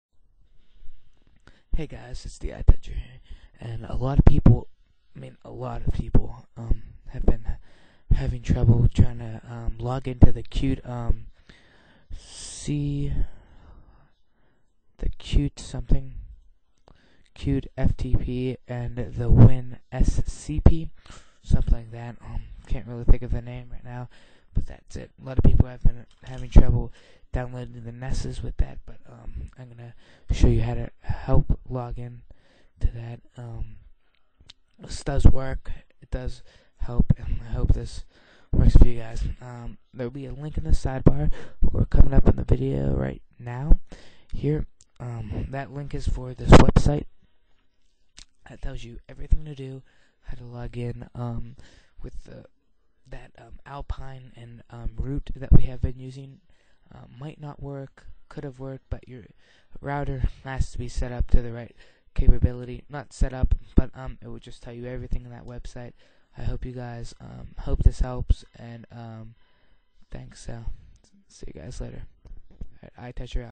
How to log in to Cute FTP and WinScp REALLY WORKS!
hey guys i just found a way on how to login to these clients Hope this helps IT should tho any question vist my site and post a message THANKS!